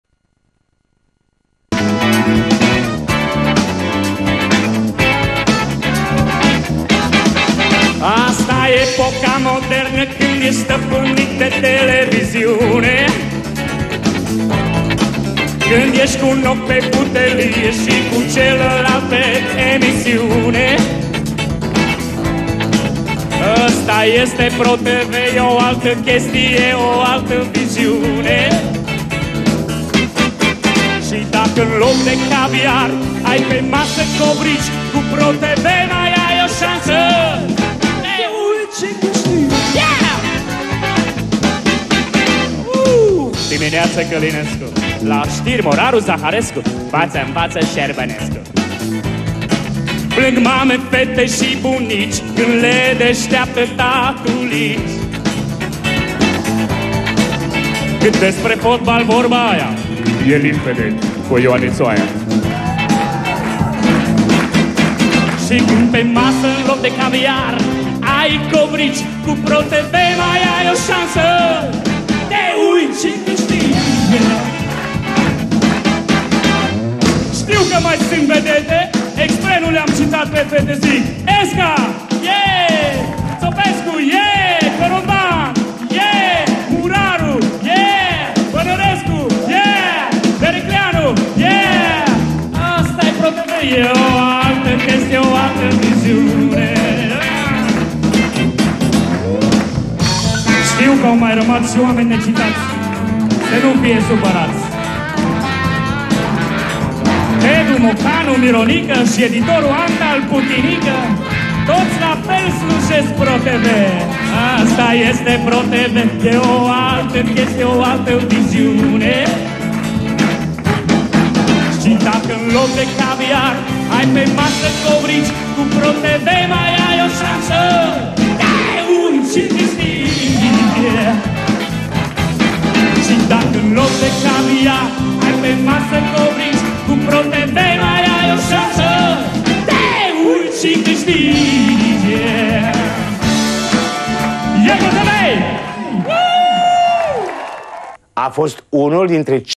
Recital Gyuri Pro TV 1997
Un concert prin care Gyuri omagiază activitatea vedetelor Pro Tv (Îi veți recunoaște în public) ...Cu tristețe încarc acest video pe canalul meu de YT, în semn de omagiu pentru colegul Ioan Gyuri Pascu , acest artist ”uriaș” care a dat ”sarea și piperul” grupurilor din care a făcut parte și de la a cărui trecere într-o lume mai bună se împlinesc astăzi , 25 septembrie 2023, fix 7 ani ...Dumnezeu să-l odihnească în pace și să-i înalțe Lumina ! ...